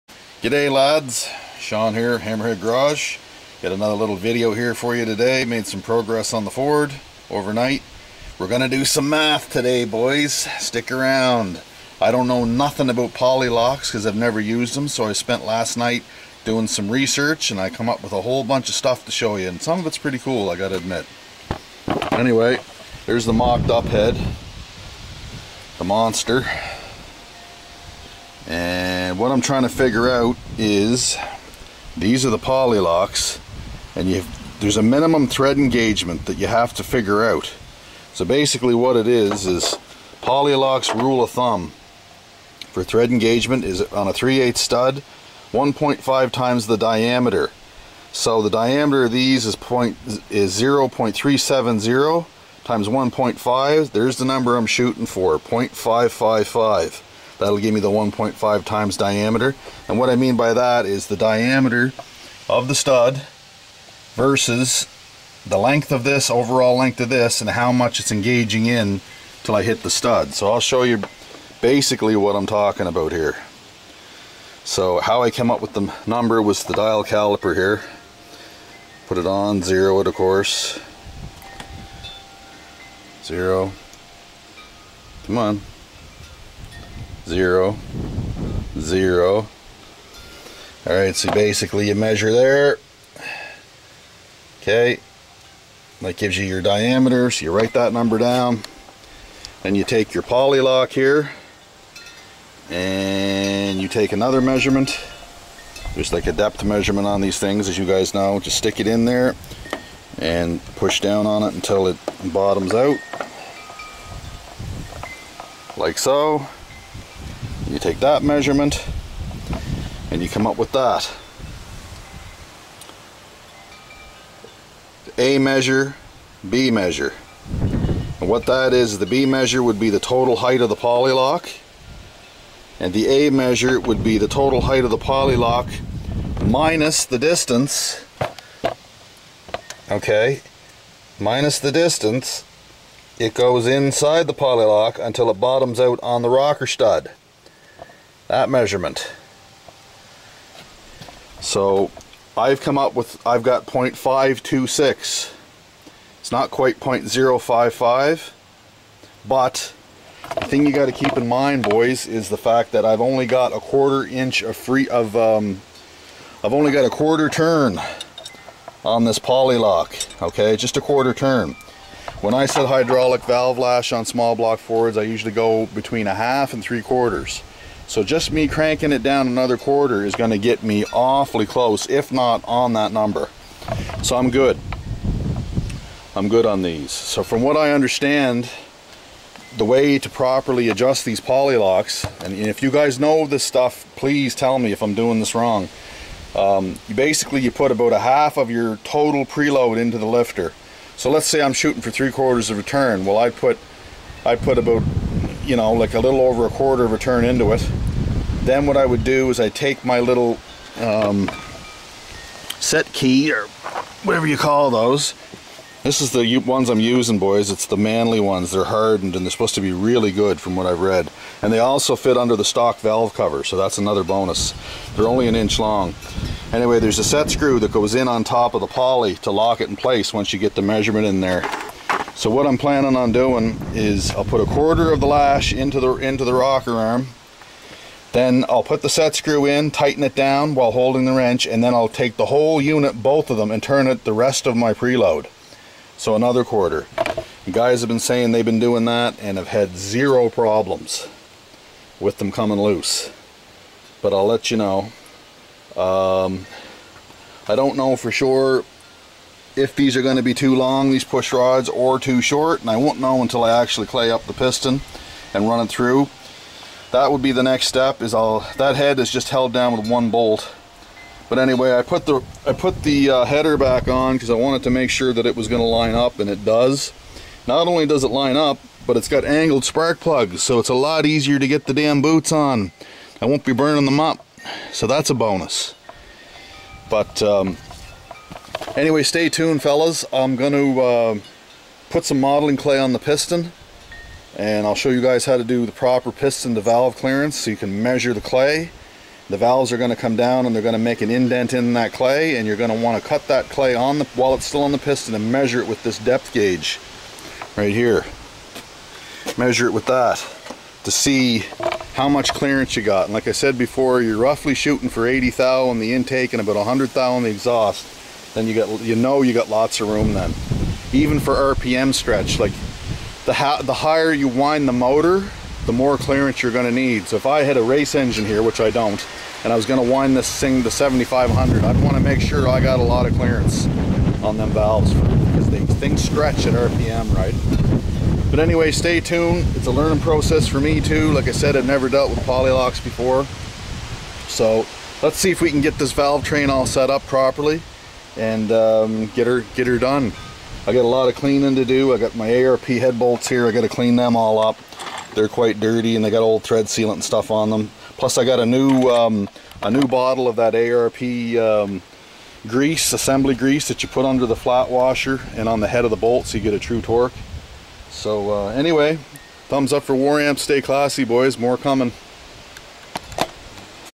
Engine math and valve train setup SBF Part 2
We dive into it and do some math to make sure things are correct! Thanks for being part of the journey!